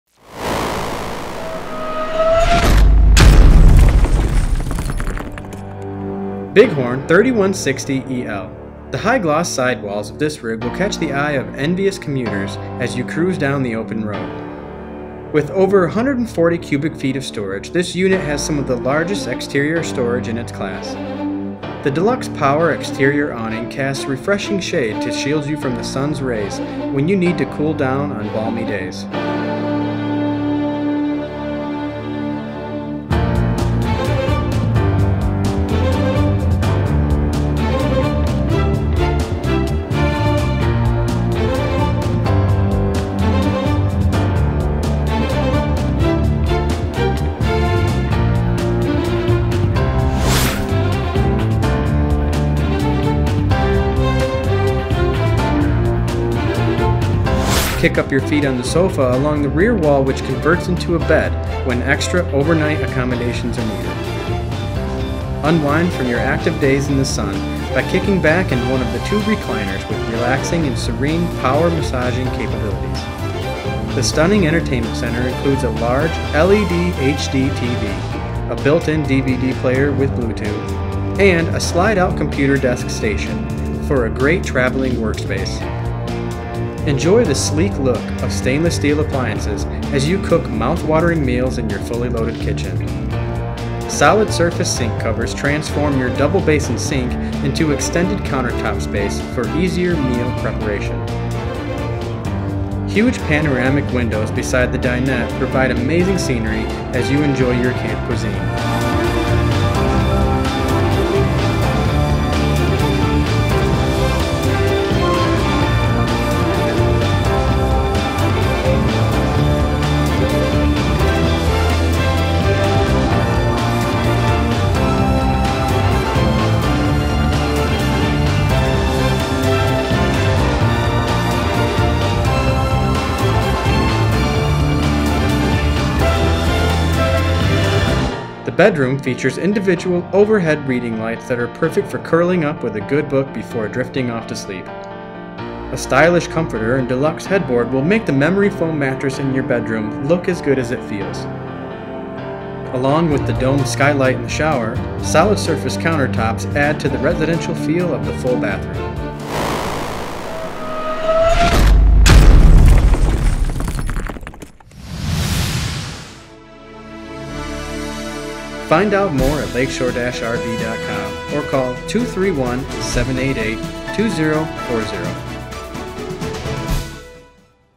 2017 Heartland Bighorn 3160EL Fifth Wheel Lakeshore RV
Heartland Bighorn 3160EL

The high gloss sidewalls of this rig will catch the eye of envious commuters as you cruise down the open road.

With over 140-cubic-feet of storage, this unit has some of the largest exterior storage in its class!

The deluxe power exterior awning casts refreshing shade to shield you from the sun’s rays when you need to cool down on balmy days.

Kick up your feet on the sofa along the rear wall which converts into a bed when extra overnight accommodations are needed.

Unwind from your active days in the sun by kicking back in one of the two recliners with relaxing and serene power massaging capabilities!

The stunning entertainment center includes a large LED HD TV, a built-in DVD player with Bluetooth, and a slide-out computer desk station for a great traveling work space!

Enjoy the sleek look of stainless steel appliances as you cook mouthwatering meals in your fully-loaded kitchen!

Solid surface sink covers transform your double-basin sink into extended countertop space for easier meal preparation.

Huge panoramic windows beside the dinette provide amazing scenery as you enjoy your camp cuisine.

The bedroom features individual overhead reading lights that are perfect for curling up with a good book before drifting off to sleep.

A stylish comforter and deluxe headboard will make the memory foam mattress in your bedroom look as good as it feels!

Along with the domed skylight in the shower, solid surface countertops add to the residential feel of the full bathroom.